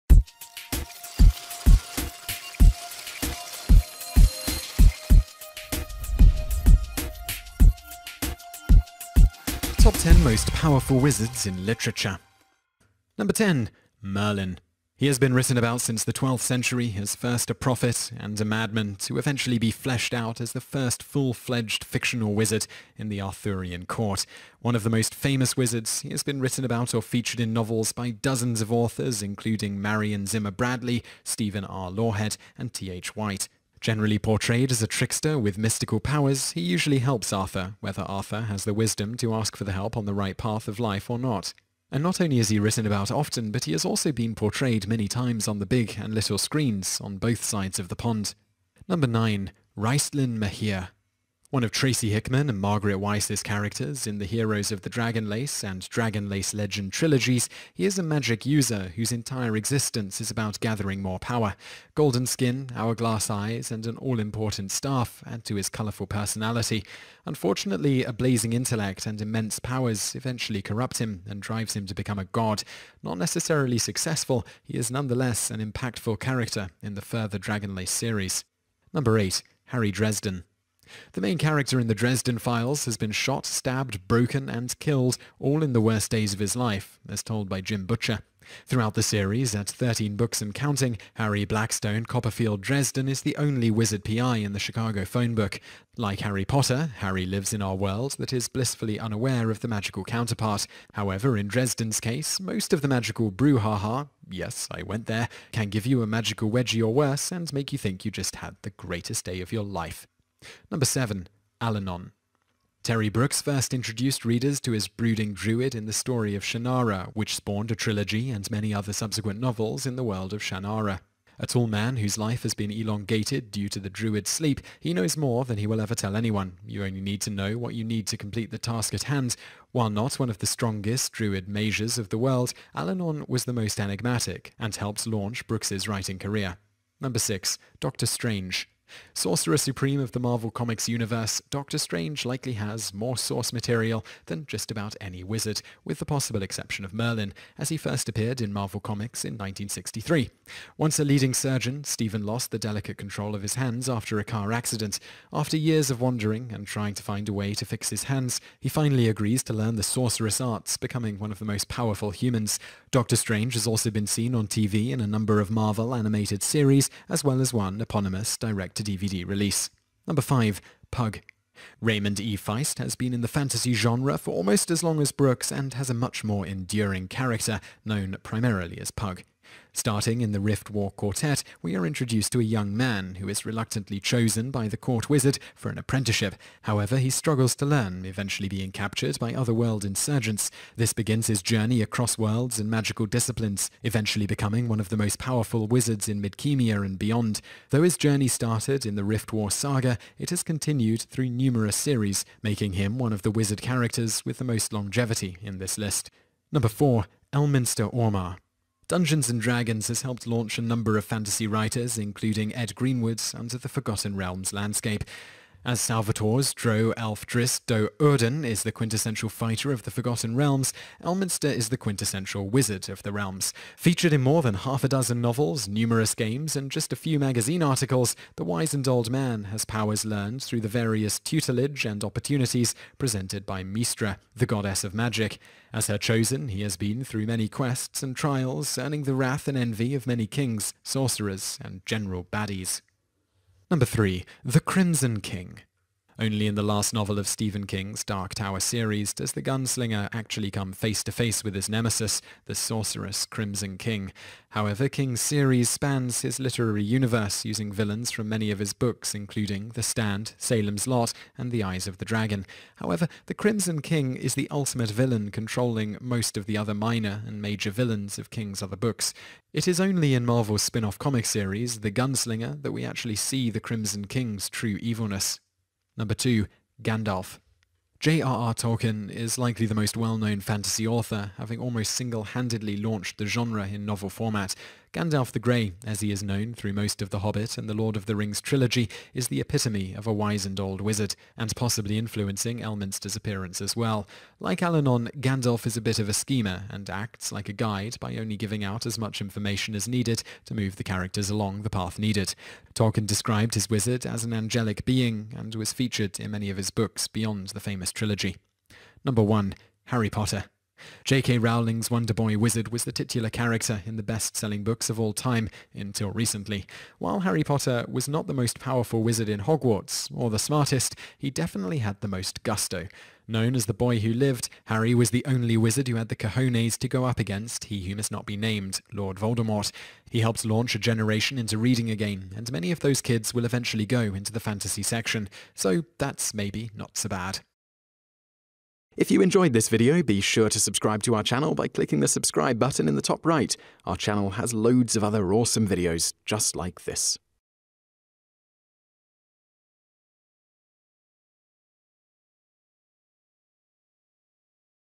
Top 10 Most Powerful Wizards in Literature — TopTenzNet
Top 10 Most Powerful Wizards in Literature

Top 10 People Who Were Too Pretty For Their Own Good

In the last century, entertainment has taken huge leaps forward with the advent of first radio, then television, movies and the Internet. However, one form of entertainment has evolved – writing, specifically novels and the long form of fiction. Funny thing about that evolution is that the fiction genre of fantasy has only just started growing in popularity over the last 30 years or so, despite the best efforts of authors like J.R.R. Tolkien. The history and fascination with fantasy has been around for centuries (think fairy tales and the Brothers Grimm), but only with modern publication and the acceptance of the geek culture has fantasy really surged in popularity. In that time, there have been at least 10 really excellent wizards and sorcerers to strut their stuff across the literary landscape – and here they are in no particular order.

Coming up:

10. Merlin
9. Raistlin Majere
8. Harry Dresden
7. Allanon
6. Dr. Strange
5. Pug
4. Elminster Aumar
3. The Crimson King
2. Gandalf
1. Harry Potter